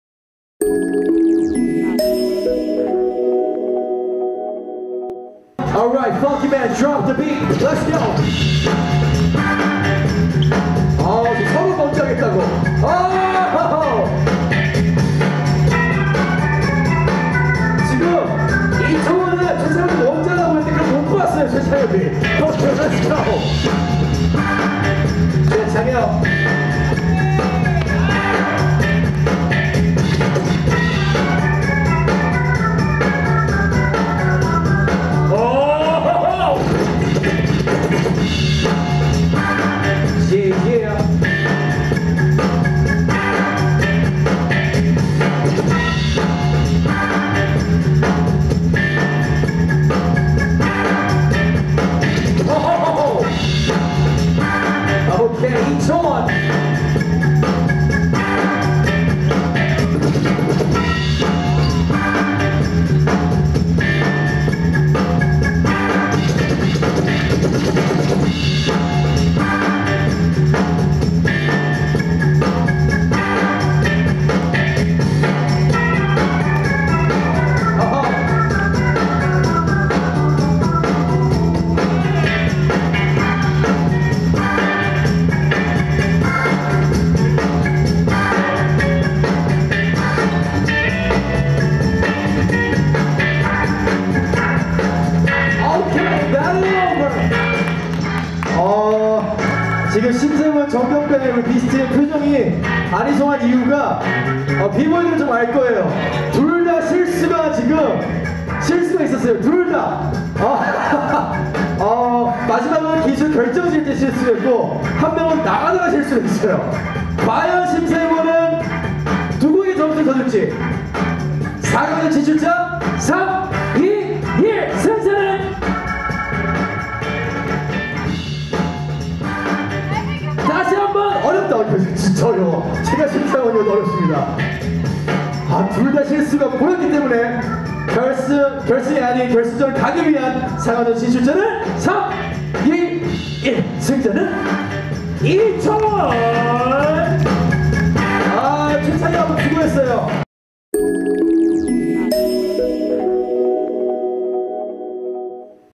[싹튜브] 서종예 서울종합예술실용학교 스트릿댄스대회 비보이 8강 최창협 이초원 배틀
비보이 8강 최창협 이초원 배틀